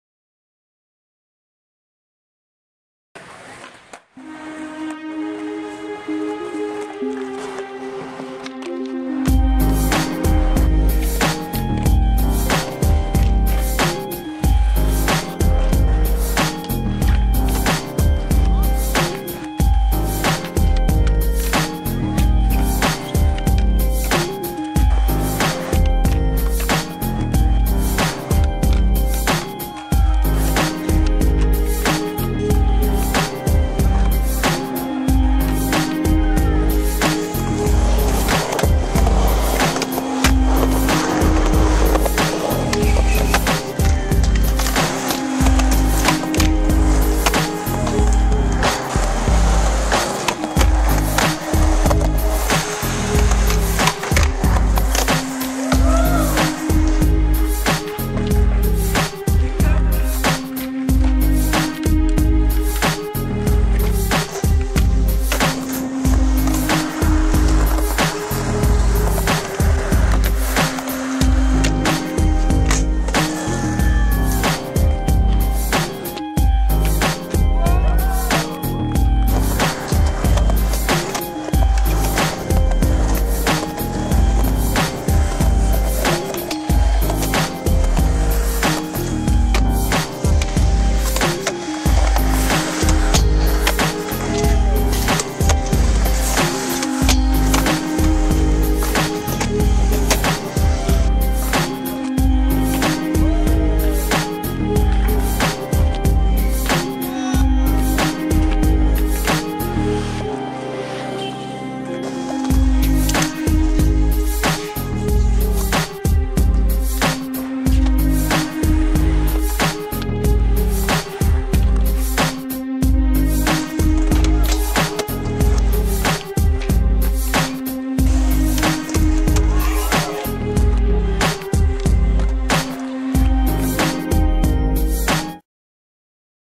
West Covina Montage
West Covina park montage featuring: Jacob Smith, Doug Des Autels, Sean Grohs, Anthony Sanchez, Martin Bernal, & Eljay McLucas